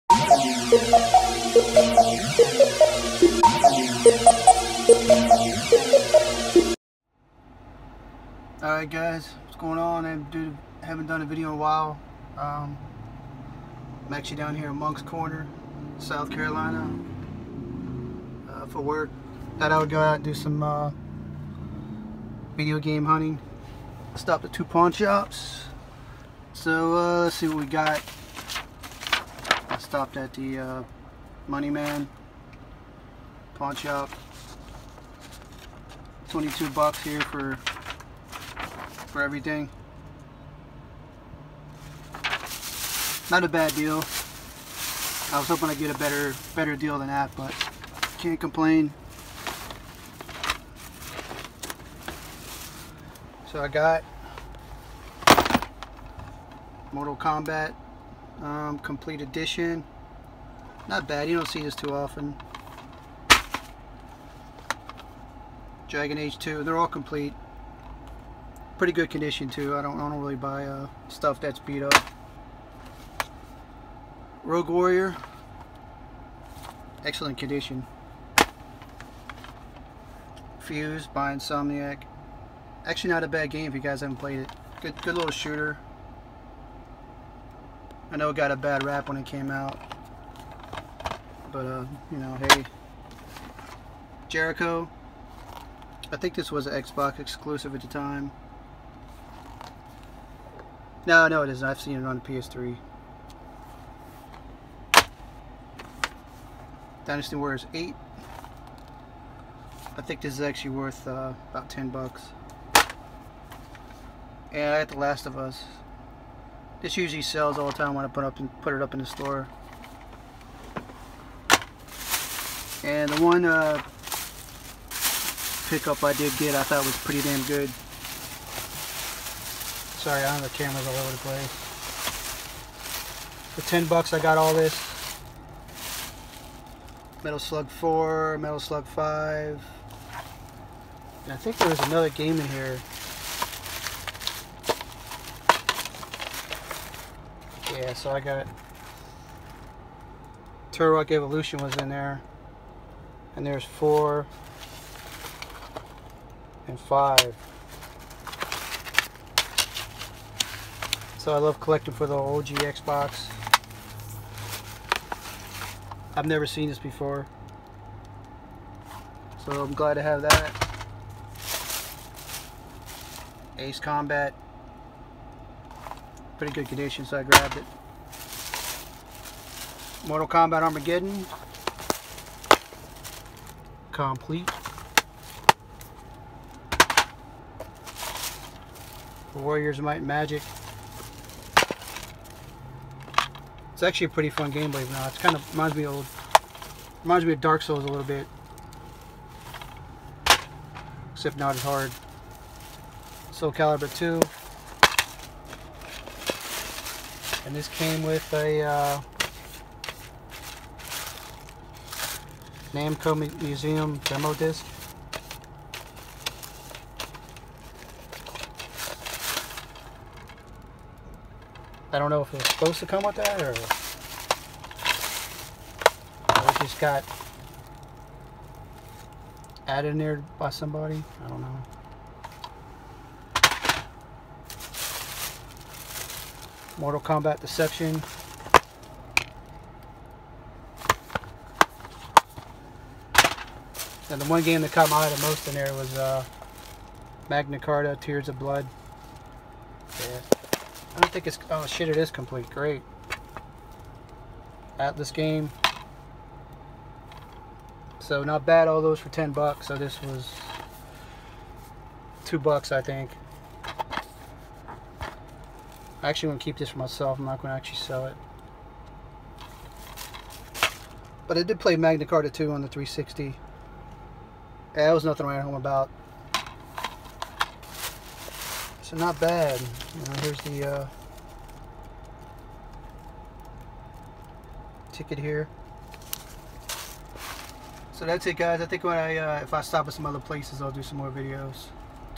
Game hunting in South Carolina
looking for inventory for my eBay store.